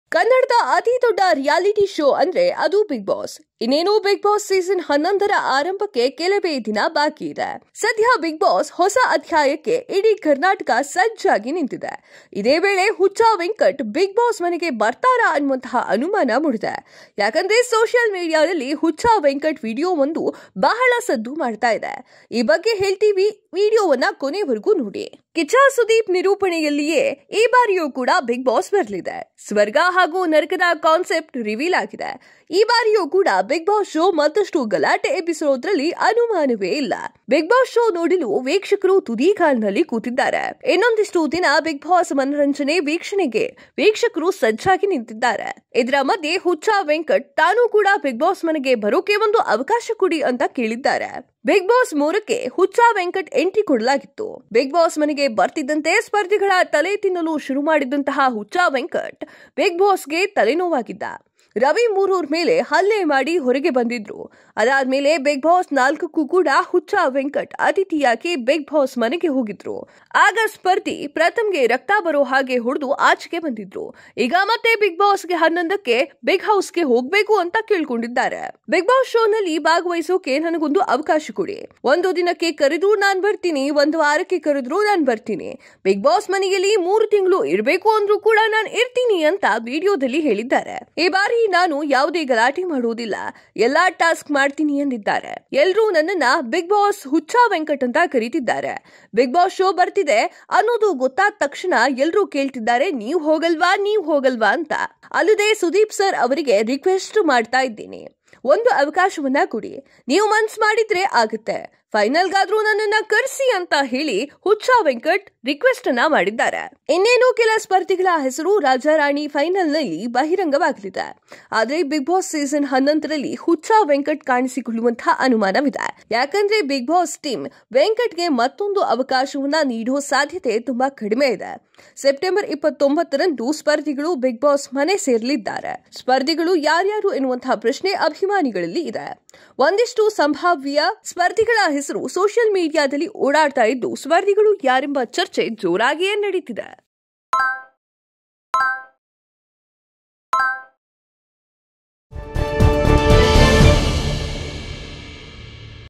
ಬಿಗ್ ಬಾಸ್​​ಗೆ ಹುಚ್ಚ ವೆಂಕಟ್ | ಈ ಬಾರಿ ಹಿಂಗೆ ಮಾಡಲ್ವಂತೆ! | Huccha Venkat | Bigg Boss | Karnataka TV News
ಕನ್ನಡದ ಅತೀ ದೊಡ್ಡ ರಿಯಾಲಿಟಿ ಶೊ ಅಂದ್ರೆ ಅದು ಬಿಗ್ ಬಾಸ್. ಇನ್ನೇನು ಬಿಗ್ ಬಾಸ್ ಸೀಸನ್ 11 ರ ಆರಂಭಕ್ಕೆ ಕೇವಲ ಒಂದೇ ದಿನ ಬಾಕಿ ಇದೆ.   ಸದ್ಯ ಬಿಗ್ ಬಾಸ್ ಹೊಸ ಅಧ್ಯಾಯಕ್ಕೆ ಇಡೀ ಕರ್ನಾಟಕ ಸಜ್ಜಾಗಿದೆ. ಇದೇ ವೇಳೆ ಹುಚ್ಚ ವೆಂಕಟ್ ಬಿಗ್ ಮನೆಗೆ ಬರ್ತಾರಾ ಅನ್ನೋ ಅನುಮಾನ ಮೂಡಿದೆ.  ಯಾಕಂದ್ರೆ ಸೊಶೀಯಲ್ ಮೀಡಿಯಾದಲ್ಲಿ ಹುಚ್ಚ ವೆಂಕಟ್ ವೀಡಿಯೋವೊಂದು ಬಹಳ ಸದ್ದು ಮಾಡ್ತಿದೆ ಆ ಬಗ್ಗೆ ಹೇಳ್ತೀವಿ ಇಂದಿನ ವೀಡಿಯೋ ದಲ್ಲಿ .

ಕಿಚ್ಚ ಸುದೀಪ್ ನಿರೂಪಣೆಯಲ್ಲಿಯೇ ಈ ಬಾರಿಯೂ ಬಿಗ್ ಬಾಸ್ ಬರಲಿದೆ.  ಸ್ವರ್ಗ ಮತ್ತು ನರಕ ಕಾನ್ಸೆಪ್ಟ್  ರೀವಿಲ್ ಆಗಿದೆ.  ಈ ಬಾರಿಯೂ ಬಿಗ್ ಬಾಸ್  ಶೋ ಮತ್ತಷ್ಟು ಗಲಾಟೆ ಎಬ್ಬಿಸೋದ್ರಲ್ಲಿ ಅನುಮಾನವಿಲ್ಲ. ಬಿಗ್ ಬಾಸ್ ಶೋ ನೋಡಲು ವೀಕ್ಷಕರು ತುದಿಗಾಲಿನಲ್ಲಿ ಕುಳಿತಿದ್ದಾರೆ. ಇನ್ನೊಂದಿಷ್ಟು ದಿನ ಬಿಗ್ ಬಾಸ್ ಮನರಂಜನೆ ವೀಕ್ಷಕರಿಗೆ ಸಿಗಲಿದೆ.  ಇದರ ಮದ್ಯೆ ಹುಚ್ಚ ವೆಂಕಟ್ ತಾನೂ ಬಿಗ್ ಮನೆಗೆ ಬರೋಕೆ ಅವಕಾಶ ಕೊಡಿ ಅಂದಿದ್ದಾರೆ.

ಬಿಗ್ ಬಾಸ್ 3ಕ್ಕೆ  ಹುಚ್ಚ ವೆಂಕಟ್ ಎಂಟ್ರಿ ಕೊಟ್ಟಿದ್ರು.  ಬಿಗ್ ಮನೆಗೆ ಬರ್ತಿದ್ದಂತೆ ಸ್ಪರ್ಧಿಗಳ ತಲೆ ತಿನ್ನಲು ಶುರು ಮಾಡಿದ್ದ ಹುಚ್ಚ ವೆಂಕಟ್ ಬಿಗ್ ಬಾಸ್ ಗೆ ತಲೆನೋವಾಗಿದ್ದ.   ರವಿ ಮೂರೂರ್ ಮೇಲೆ ಹಲ್ಲೆ ಮಾಡಿ ಹೊರಗೆ ಬಂದಿದ್ದರು. ಅದಾದ್ಮೇಲೆ  ಬಿಗ್​ ಬಾಸ್ 4ಕ್ಕೂ ಹುಚ್ಚ ವೆಂಕಟ್ ಅತಿಥಿಯಾಗಿ ಬಿಗ್ ಬಾಸ್ ಮನೆಗೆ ಹೋಗಿದ್ರು.. ಆಗ ಸ್ಪರ್ಧಿ ಪ್ರಥಮ್​​ಗೆ ರಕ್ತ ಬರುವಂತೆ ಹೊಡೆದು ಆಚೆ ಬಂದಿದ್ರು.. ಈಗ ಮತ್ತೆ ಬಿಗ್ ಬಾಸ್ 11ಕ್ಕೆ ಬಿಗ್ ಹೌಸ್ ಗೆ ಹೋಗ್ಬೇಕು ಅಂತ ಕೇಳಿಕೊಂಡಿದ್ದಾರೆ..

ಬಿಗ್ ಬಾಸ್ ಶೋನಲ್ಲಿ ಭಾಗಿಯಾಗೋಕೆ  ನನಗೆ ಒಂದು ಅವಕಾಶಕೊಡಿ.  ಒಂದು ದಿನಕ್ಕೆ ಕರೆದ್ರೂ ನಾನು ಬರ್ತೇನೆ. ಒಂದು ವಾರಕ್ಕೆ ಕರೆದ್ರೂ ನಾನು ಬರ್ತೇನೆ.   ಬಿಗ್ ಬಾಸ್ ಮನೆಯಲ್ಲಿ ಮೂರು ತಿಂಗಳು ಇರ್ಬೇಕು ಅಂದ್ರೂ ನಾನು ಇರ್ತೀನಿ ಅಂತ ವಿಡಿಯೋ ಮಾಡಿದ್ದಾರೆ.. ಈ ಬಾರಿ ನಾನು ಯಾವುದೇ ಗಲಾಟೆ ಮಾಡೋಲ್ಲಾ, ಎಲ್ಲಾ ಟಾಸ್ಕ್ ಮಾಡ್ತೀನಿ ಅಂದಿದ್ದಾರೆ.  ಎಲ್ಲರೂ ನನ್ನನ್ನ ಬಿಗ್ ಬಾಸ್ ಹುಚ್ಚ ವೆಂಕಟ್ ಅಂತ ಕರಿತಾರೆ.  ಬಿಗ್ ಬಾಸ್ ಶೋ ಬರ್ತಿದೆ ಅನ್ನೋದು ಗೊತ್ತಾದಾಗ ಎಲ್ಲರೂ ನೀವು ಹೋಗಲ್ವಾ, ನೀವು ಹೋಗಲ್ವಾ ಅಂತ ಕೇಳ್ತಿದ್ದಾರೆ ಅಂದಿದ್ದಾರೆ.    ಅಲ್ಲದೇ ಸುದೀಪ್ ಸರ್ ಅವರಿಗೆ ರಿಕ್ವೆಸ್ಟ್, ಒಂದು ಅವಕಾಶಕೊಡಿ. ನೀವು ಮನಸ್ಸು ಮಾಡಿದ್ರೆ ಆಗುತ್ತೆ, ಫೈನಲ್ ಗಾದ್ರೂ ನನ್ನನ್ನು ಕರೆಯಿರಿ  ಅಂತಾ  ಹುಚ್ಚ ವೆಂಕಟ್ ರಿಕ್ವೆಸ್ಟ್ ಮಾಡಿಕೊಂಡಿದ್ದಾರೆ.

ಇನ್ನೇನು ಕೆಲ ಸ್ಪರ್ಧಿಗಳ ಹೆಸರು ರಾಜಾ ರಾಣಿ ಫೈನಲ್ ನಲ್ಲಿ ಬಹಿರಂಗವಾಗಲಿದೆ.  ಆದ್ರೆ ಬಿಗ್ ಬಾಸ್ ಸೀಸನ್ 11ರಲ್ಲಿ ಹುಚ್ಚ ವೆಂಕಟ್ ಕಾಣಿಸಿಕೋಳ್ಳೊದು ಅನುಮಾನ.   ಯಾಕಂದ್ರೆ  ಬಿಗ್ ಬಾಸ್ ಟೀಂ ವೆಂಕಟ್ ಗೆ  ಮತ್ತೊಂದು ಅವಕಾಶ ನೀಡೋ ಸಾಧ್ಯತೆ ತುಂಬಾ ಕಡಿಮೆ ಇದೆ. ಸೆಪ್ಟೆಂಬರ್ 29ರಂದು ಸ್ಪರ್ಧಿಗಳು ಬಿಗ್ ಬಾಸ್ ಮನೆ ಸೇರಲಿದ್ದಾರೆ.    ಸ್ಪರ್ಧಿಗಳು ಯಾರ್ಯಾರು ಎಂಬ ಪ್ರಶ್ನೆ ಅಭಿಮಾನಿಗಳಲ್ಲಿದೆ. ಒಂದಿಷ್ಟು ಸಂಭಾವ್ಯ ಸ್ಪರ್ಧಿಗಳ ಹೆಸರು ಸೋಶಿಯಲ್ ಮೀಡಿಯಾದಲ್ಲಿ ಓಡಾಡ್ತಿದ್ದು, ಸ್ಪರ್ಧಿಗಳು  ಯಾರೆಂಬ ಚರ್ಚೆ ಜೋರಾಗಿದೆ.